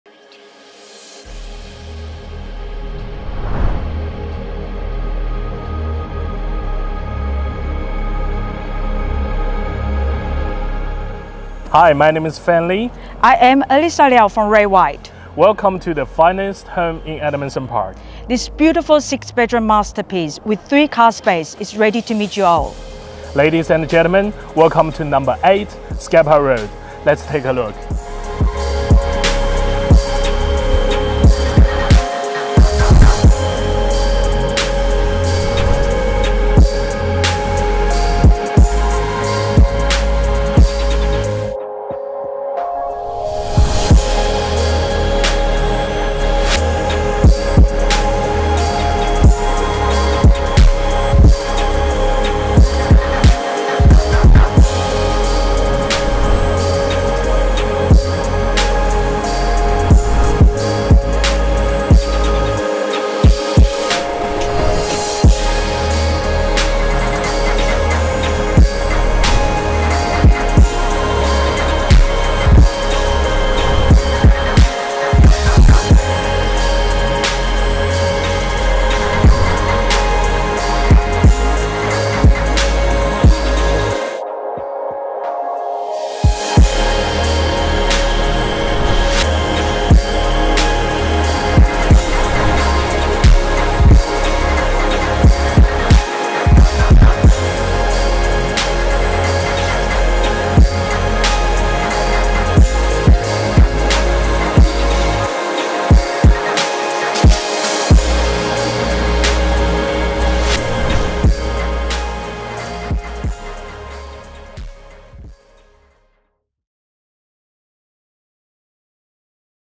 8 Scapa Road, Edmondson Park
This stunning newly built residence represents an easy and low maintenance lifestyle for families who love to entertain, yet still with family comfort in mind. The home showcases quality finishes throughout, versatile and intelligent use of space to maximise light with an north facing aspect along with multiple flowing entertaining areas. The functional and spacious floor plan of this immaculate home offers multiple living and entertainment areas over two levels. Surrounded by new quality homes and positioned in a peaceful cul-de-sac, the home is also conveniently located close to M5/M7 entrance, local schools, shops and the newly developed Edmondson Park train station, location does not get better than this.

Features include:
- Open plan kitchen overlooking living and dining areas with seamless transition into the outdoor entertainment area which boasts space and privacy
- Ultra-modern kitchen with butlers pantry, stainless steel appliances, gas cooktop and dishwasher
- Upstairs media/living room
- Four generous bedrooms with WIR/built-ins upstairs
- The 5th and sixth bedroom located on the ground floor which can be used as a guest room or for the extended family
- Modern main bathroom with floor-to-ceiling tiles and bathtub
- Master bedroom offers en-suite, walk-in-robe
- Tiles and floorboards throughout
- Expansive alfresco area with outdoor entertainment
- Two spacious and separate yards perfect for kids and pets
- Three lock-up garage with internal access and  & extra storage spaces